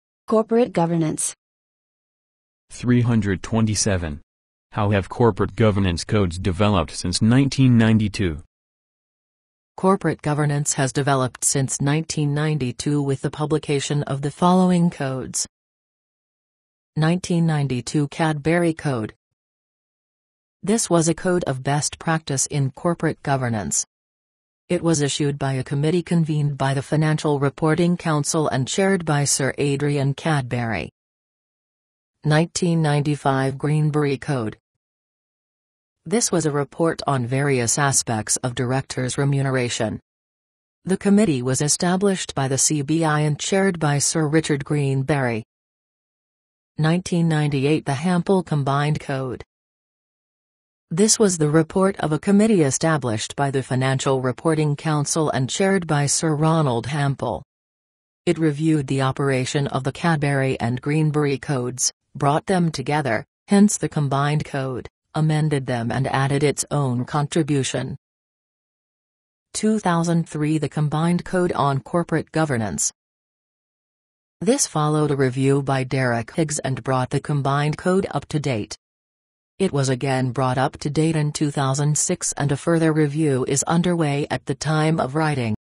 How have corporate governance codes developed since 1992?
Corporate governance has developed since 1992 with the publication of the following codes: ...